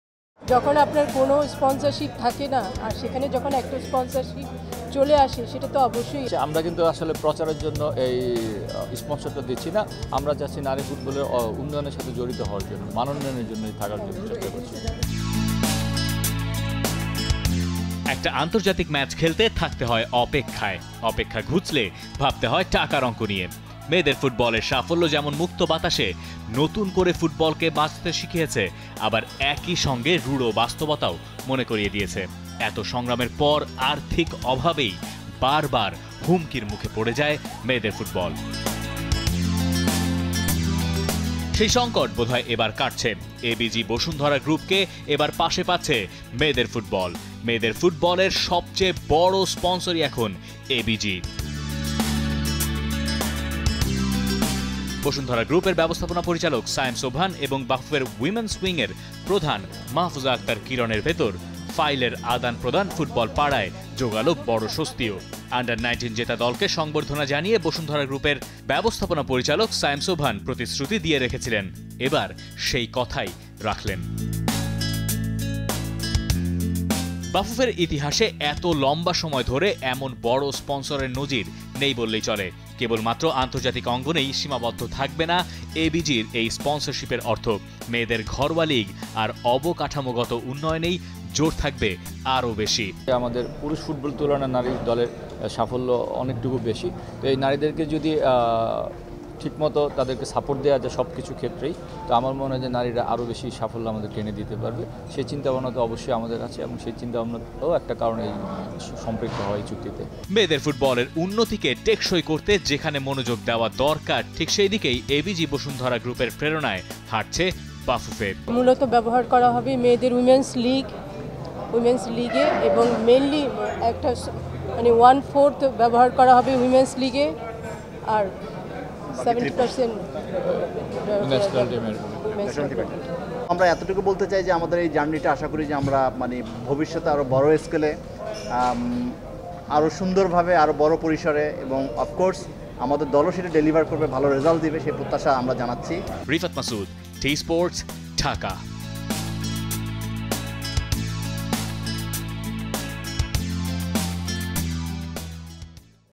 মেয়েদের ফুটবলে এবিজি বসুন্ধরার বড় স্পন্সরশিপ পেল বাফুফে
মেয়েদের ফুটবলে এবিজি বসুন্ধরার বড় স্পন্সরশিপ পেল বাফুফে | Sayem Sobhan Anvir